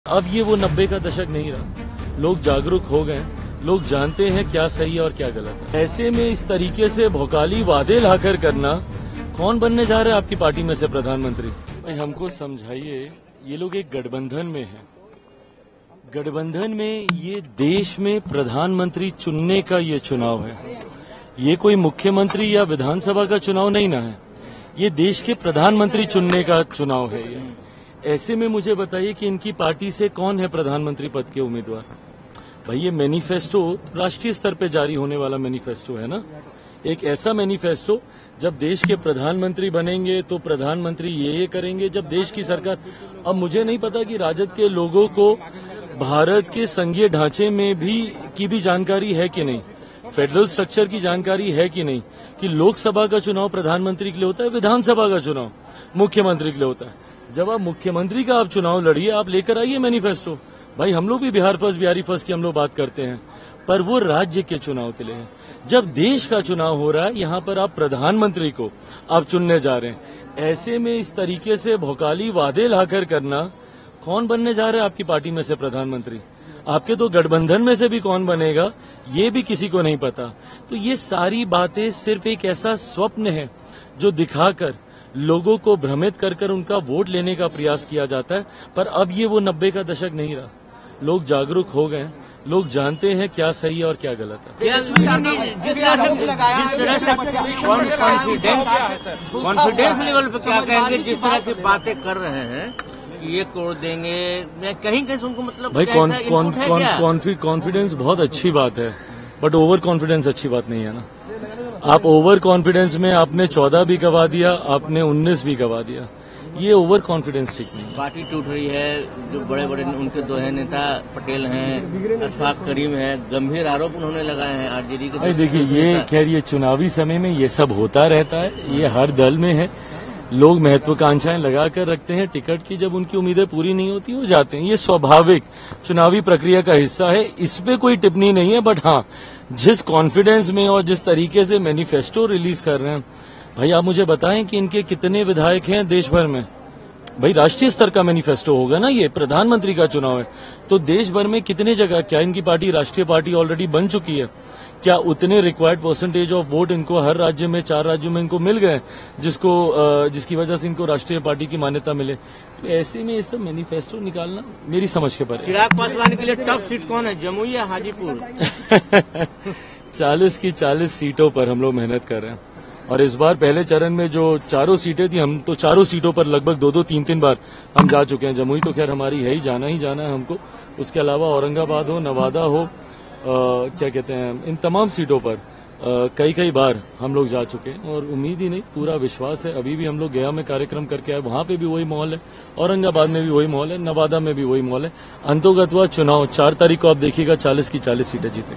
RJD के Manifesto पर Chirag Paswan ने Lalu Yadav को याद दिलाया '90 का दशक' ! | Hindi News Today
Lok Sabha Election 2024 को लेकर Lalu Yadav की पार्टी RJD ने आज अपना घोषणा पत्र जारी कर दिया है। आरजेडी के घोषणापत्र पर Chirag Paswan ने बड़ा हमला बोला है। चिराग पासवान ने कहा कि आरजेडी लोगों को भ्रमित कर रही है। ये किसी राज्य का Election नहीं बल्कि देश का Prime Minister चुनने का चुनाव है।